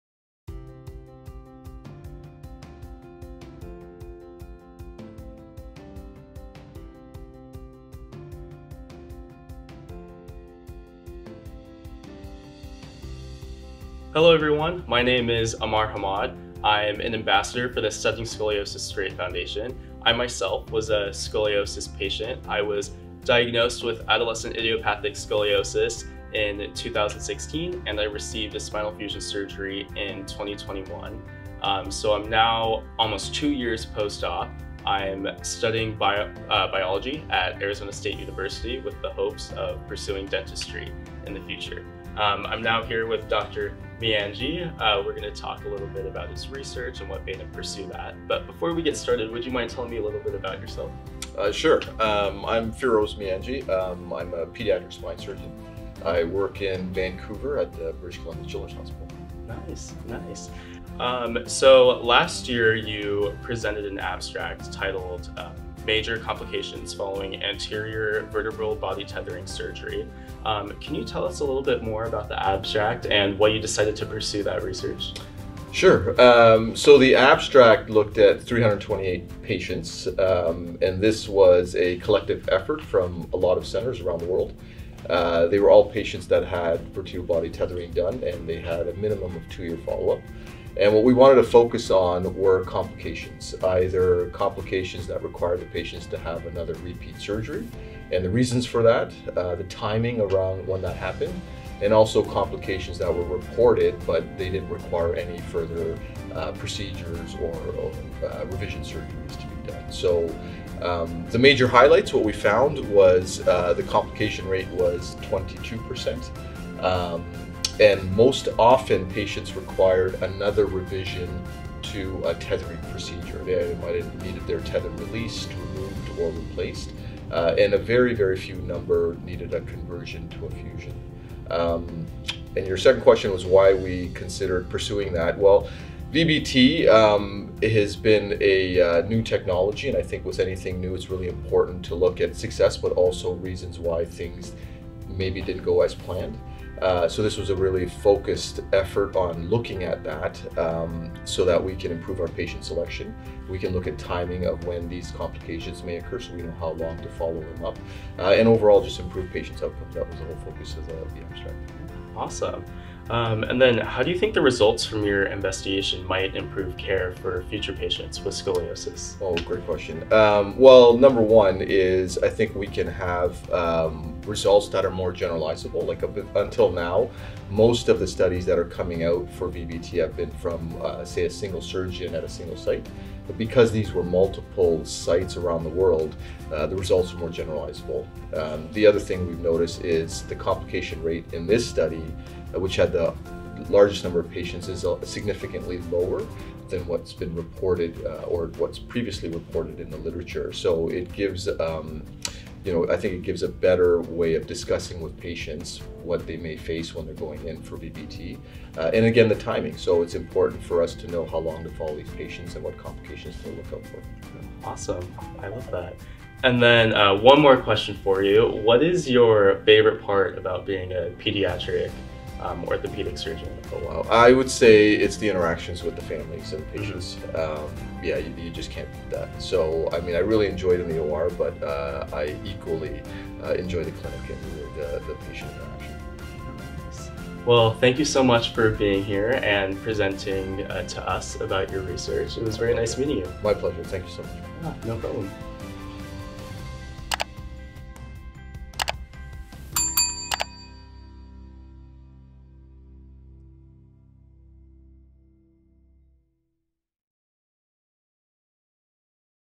Major Complications Following Anterior Vertebral Body Tethering Surgery
This study was conducted by members of the Harms Study Group.